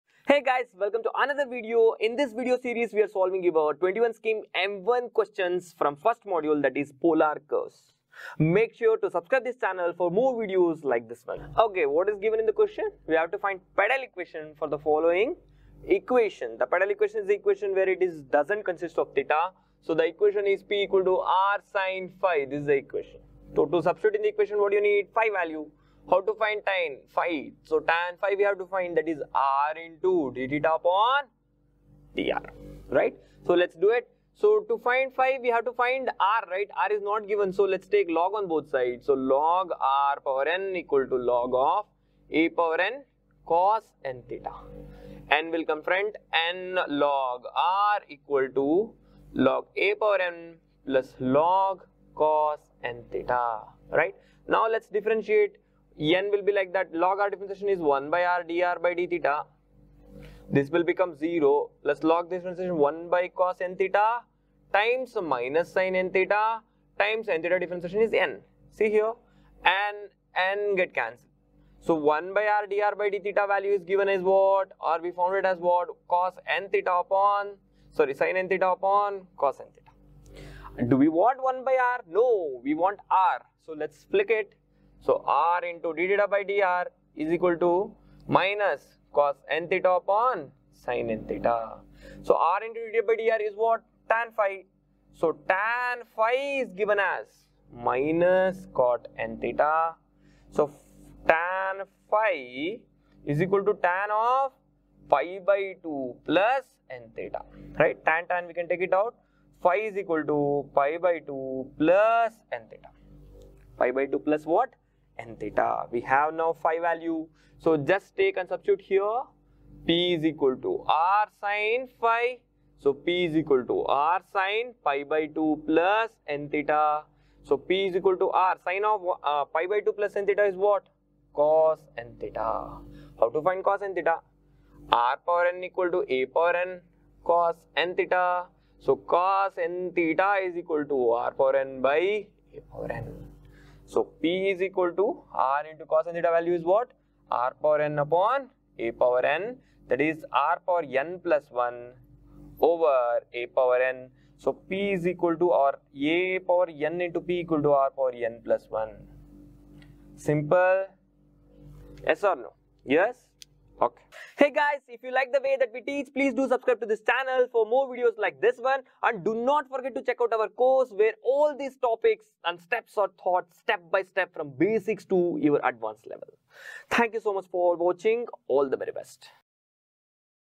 Q26 - Angle between Radius Vector and Tangent | 1st Sem VTU Math - Polar Curves
This video series covers Questions on the 1st sem VTU engineering maths Module 1: Polar Curves. Mainly on how to find the angle between the Radius Vector and the tangent at a point P(x,y).

VTU 1st sem Engineering Mathematics - 2022 scheme - Common to all branches.